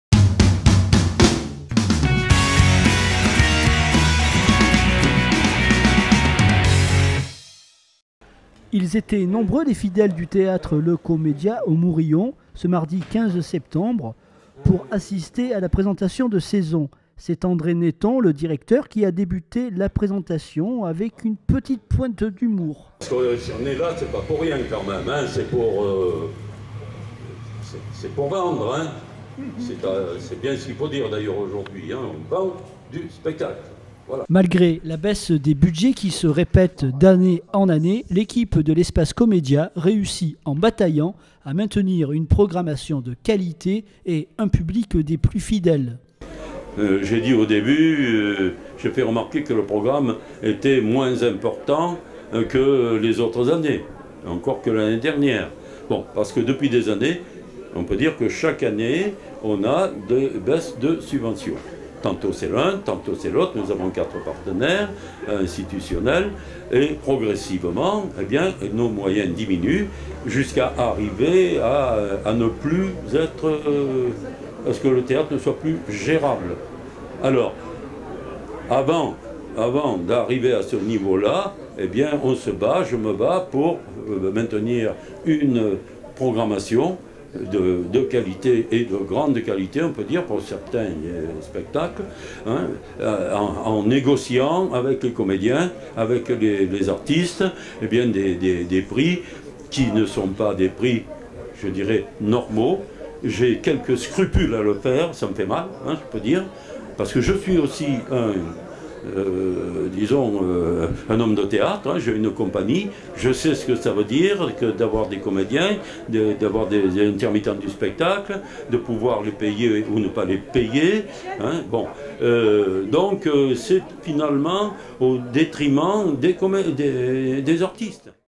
Toulon, l'Espace Comédia, bataille pour garder une programmation de qualité
L’équipe de l’Espace Comedia a invité son public à la présentation de la nouvelle programmation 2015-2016LE MARDI 15 SEPTEMBRE 2015 A 18h30en présence de plusieurs compagnies qui ont présenté leur travail.Elle s'est déroulée sur la place devant le théâtre au Mourillon à Toulon, et a été suivie d’un apéritif. L'équipe d'info83 était présente, rencontre avec André Neyton le directeur du théâtre, reportage :